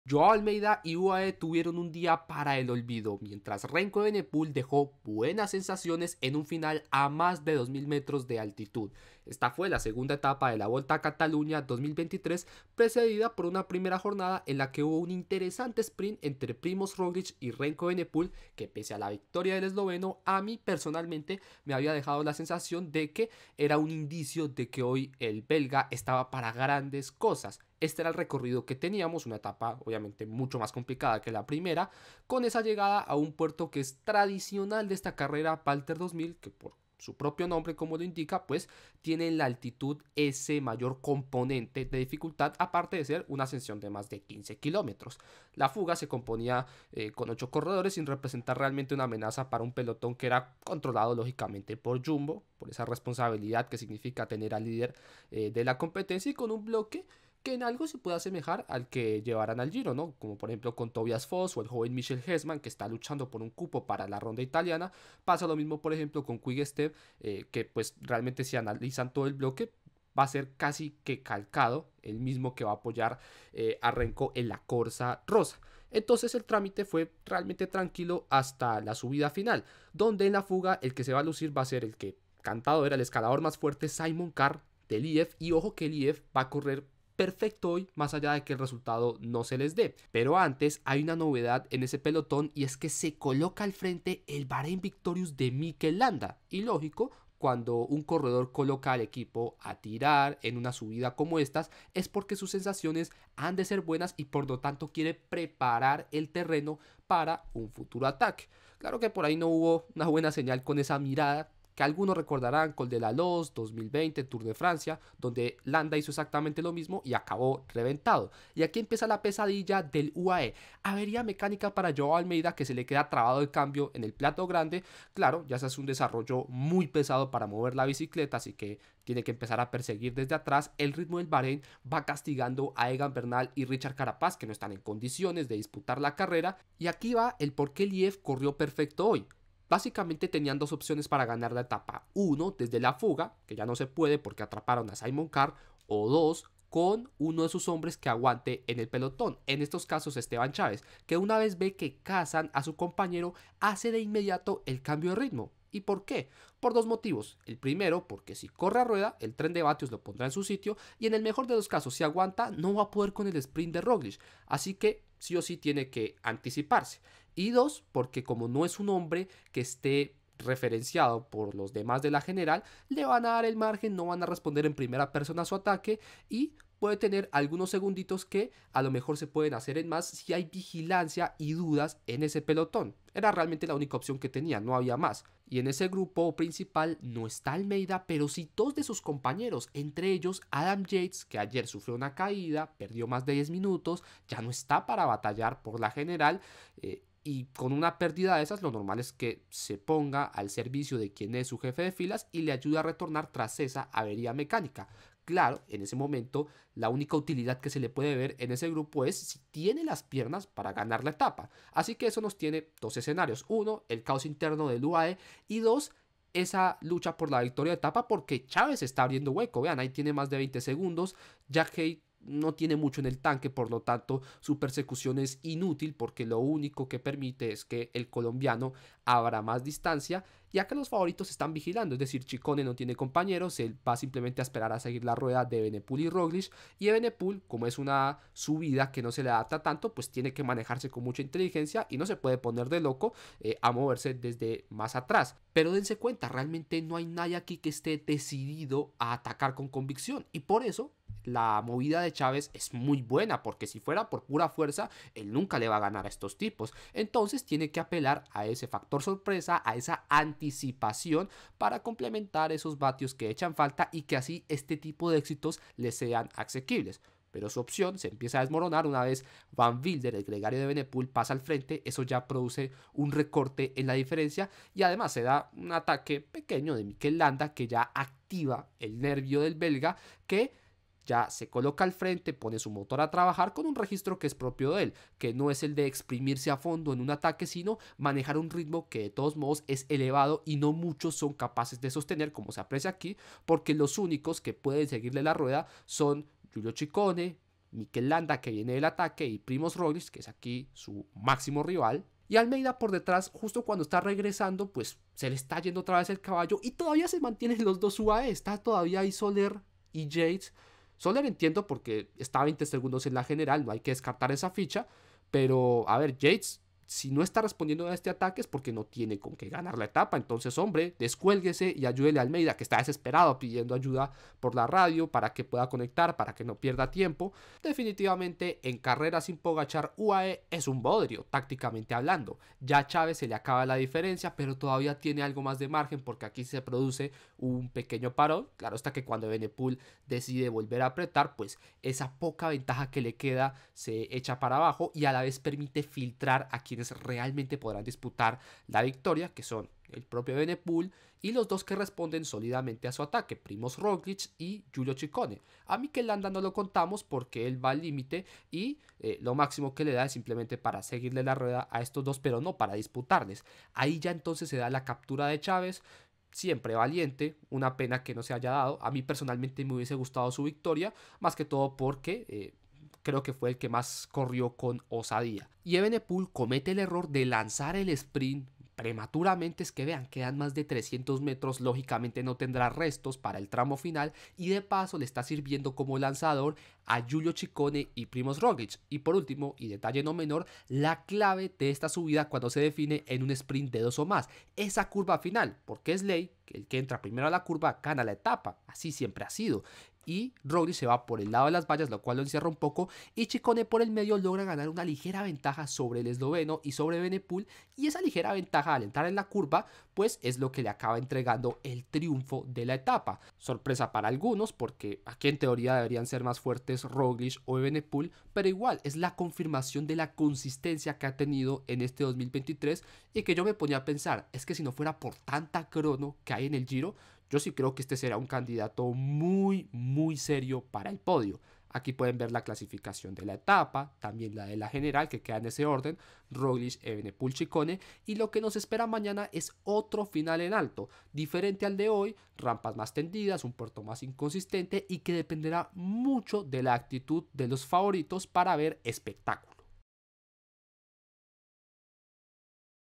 Remco Evenepoel IMPIDE ganar a Esteban Chaves | Volta a Catalunya 2023 Etapa 2
Lanterne Rouge presenta un análisis de etapa 2 de Volta a Catalunya 2023